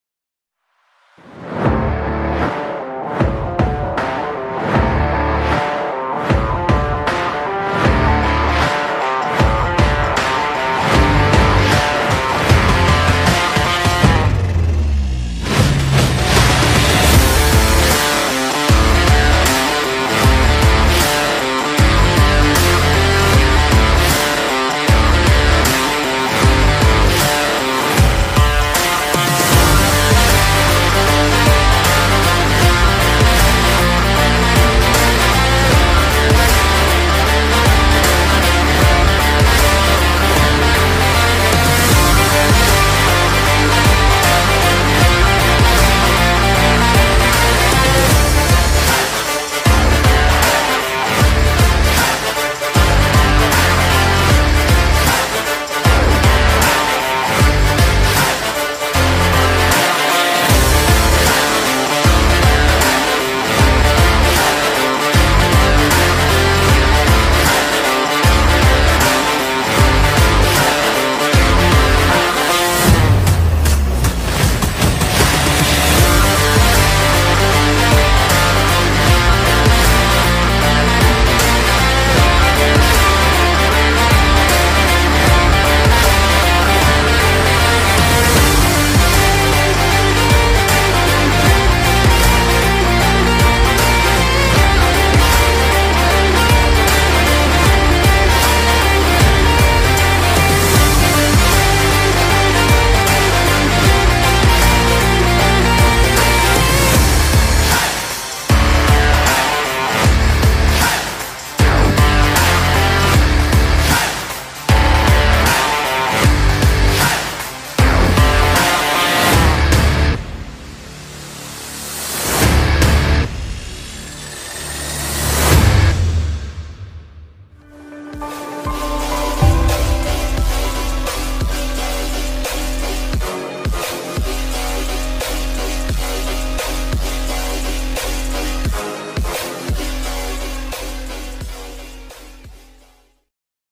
METAVERSE 2024 SUPERCAR : CUPRA COSMOS
______________. Today in this video we are going to show you all new Metaverse 2040 future concept vehicle : CUPRA COSMOS